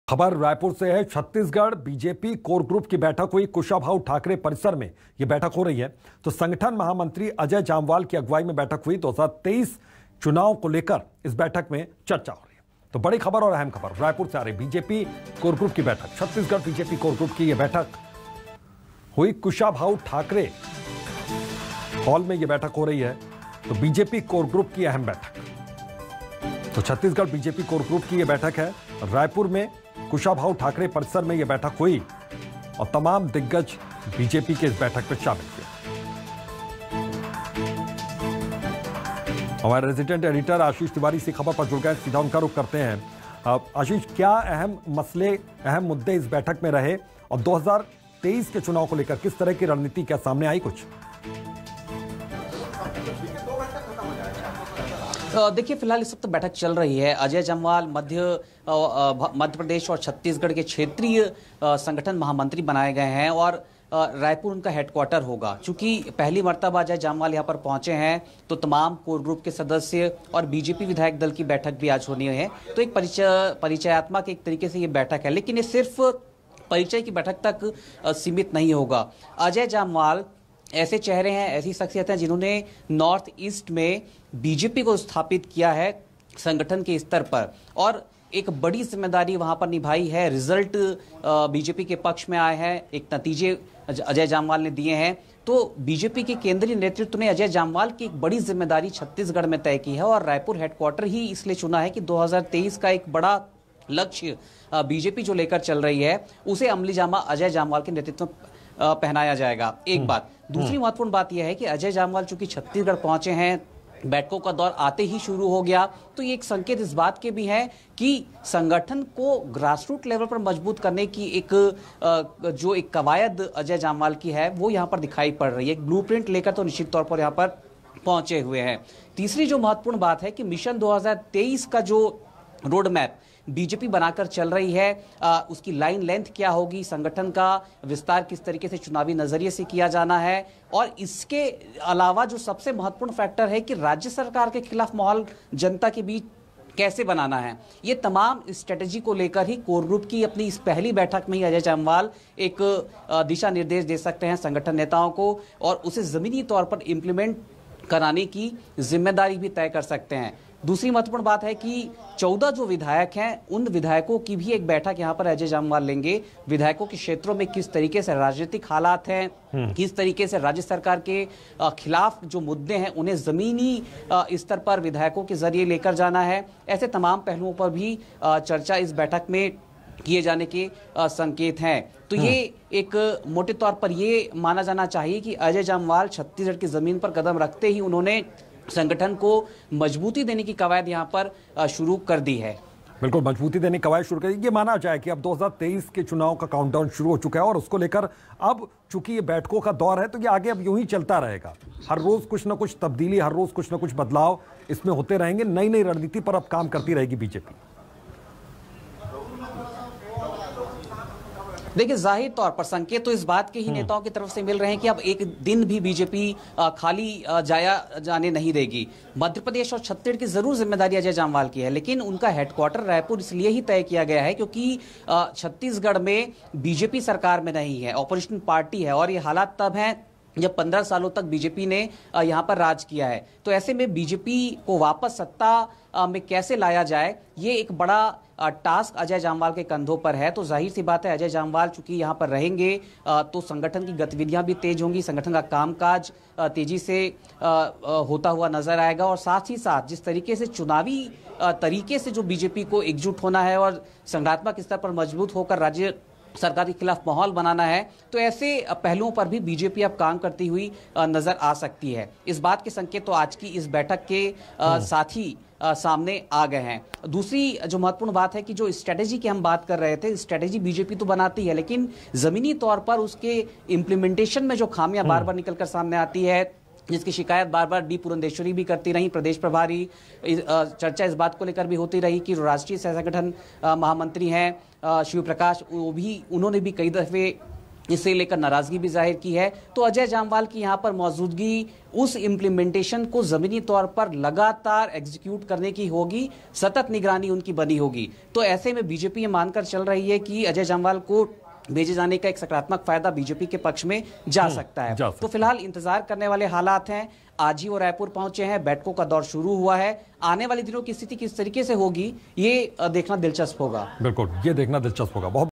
2023 चुनाव को लेकर छत्तीसगढ़ बीजेपी के कोर ग्रुप की अहम बैठक
2023 चुनाव को लेकर छत्तीसगढ़ बीजेपी के कोर ग्रुप की अहम बैठक